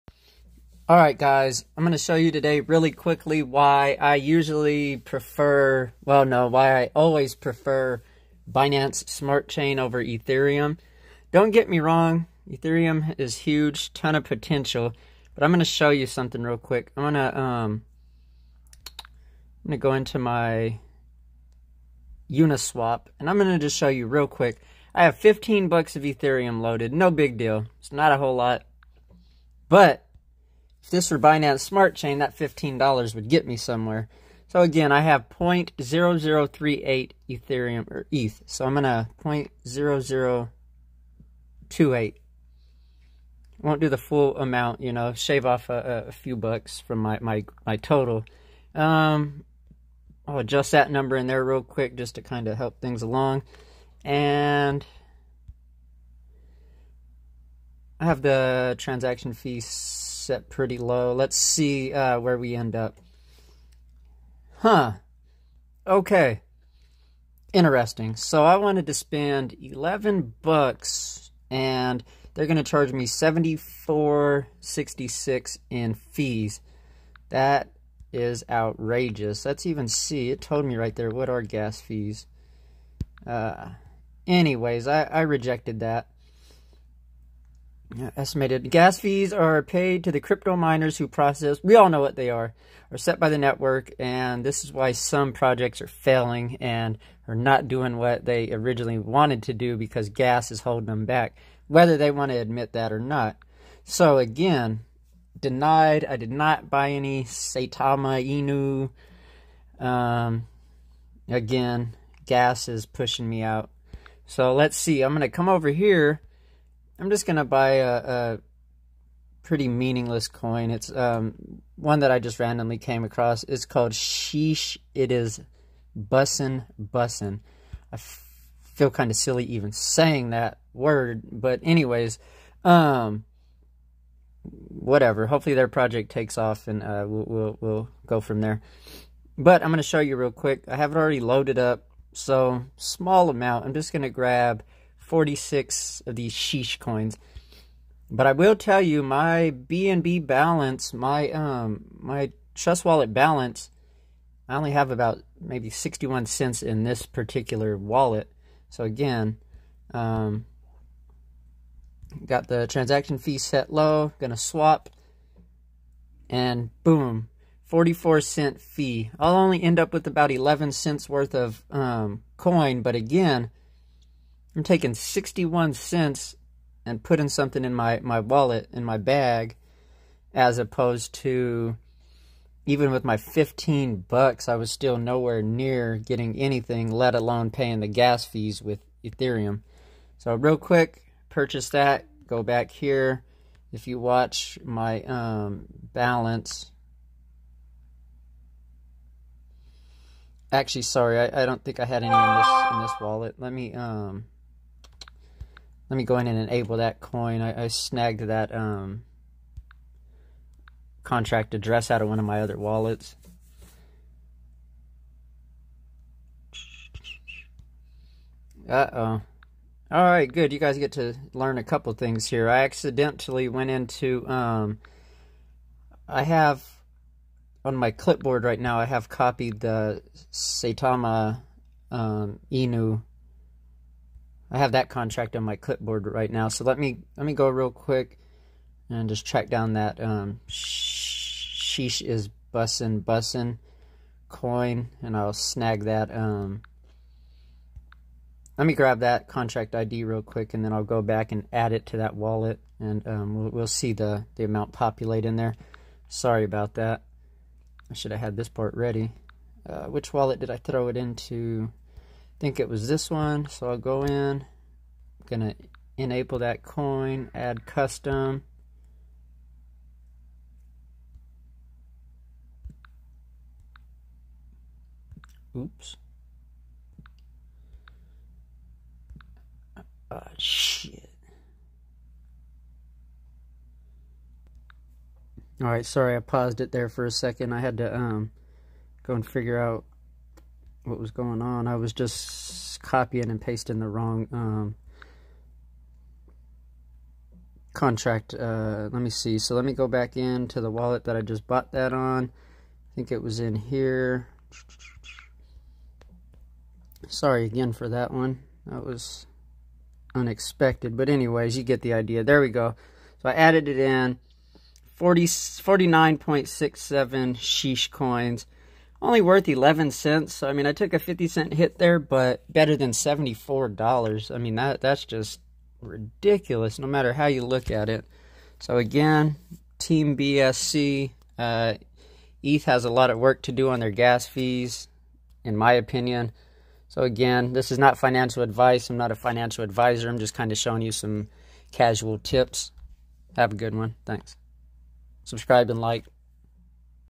Binance vs Ethereum Gas Fee's
Binance (BSC) vs Ethereum (ETH) gas Fee's!

ETH is killing it's users with gas Fee's, bsc is alot more accessible for many of us in the crypto community